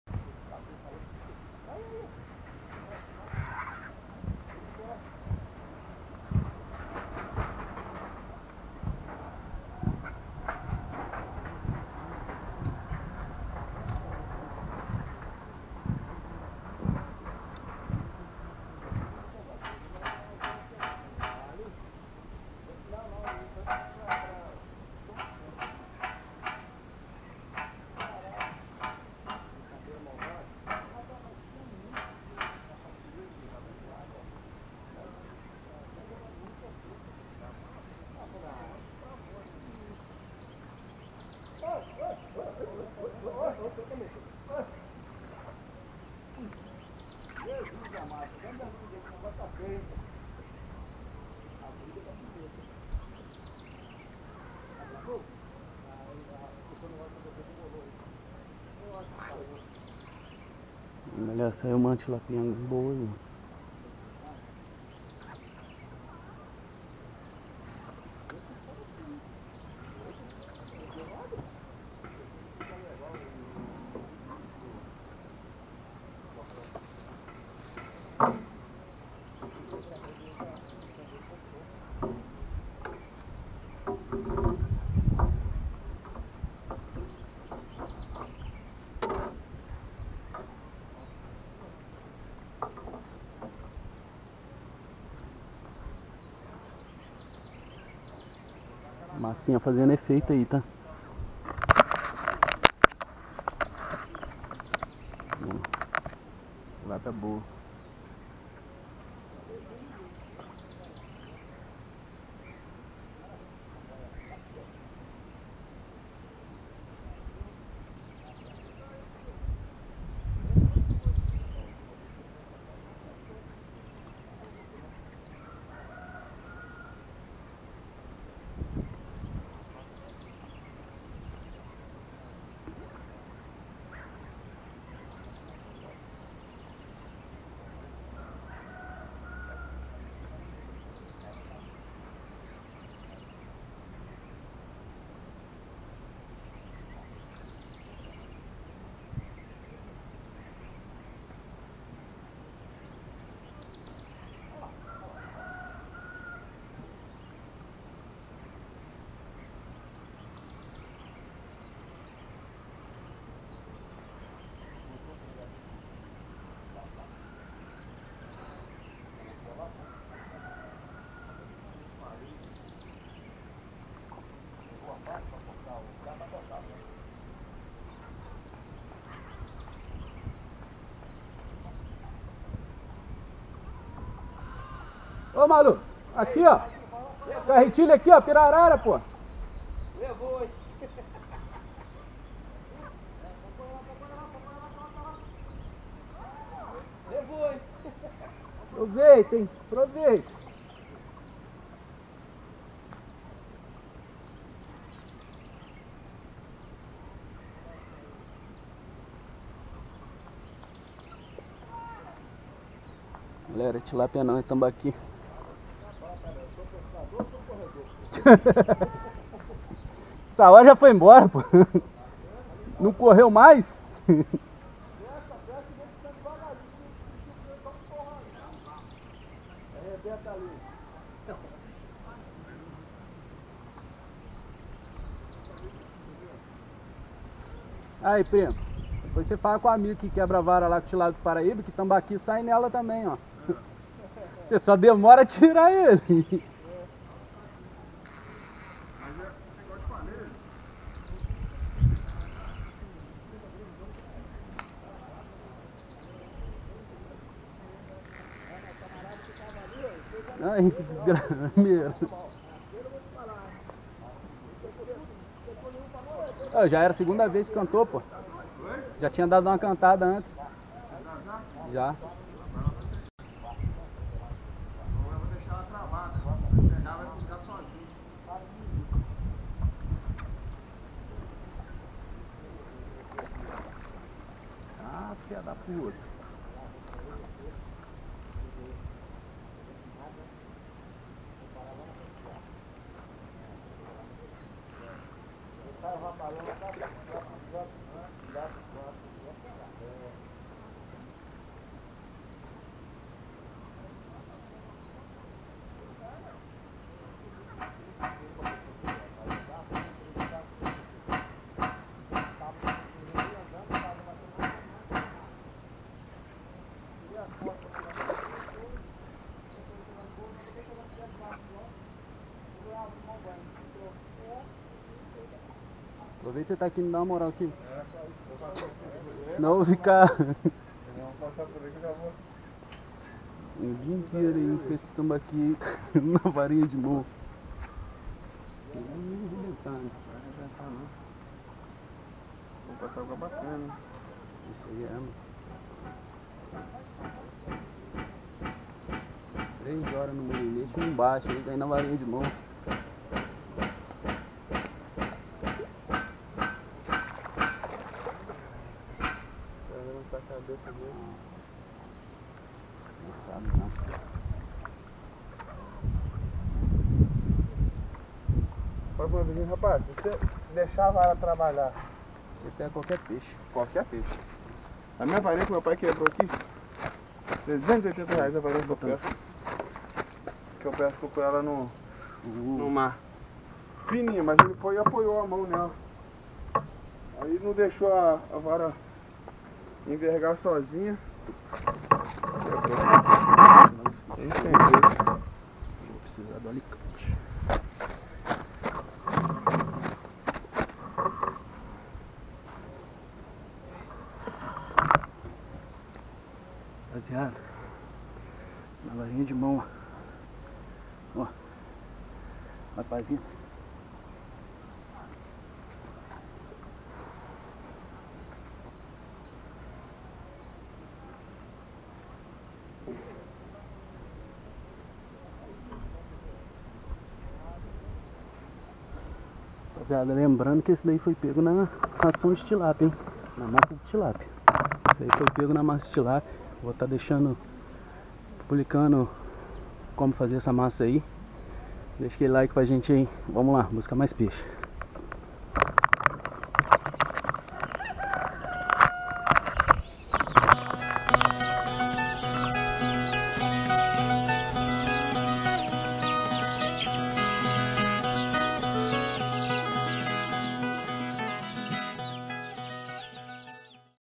EU NUNCA VI ISSO NA VARINHA DE MÃO!!
TERMINANDO NOSSA TEMPORADA NO PESQUEIRO EU NUNCA VI PEGAR UM TAMBA DESSE TAMANHO NA VARA DE MÃO
VEM COMIGO NESSA PESCARIA E DEIXE SEU COMENTARIO SE TAMBÉM  CURTE UMA PESCARIA RAIZ

pesca
pescaria
fish
fishing
fisherman
fishinglife
pesque e solte
pesca amadora
pescaria caipira
tambaqui
isca
melhor isca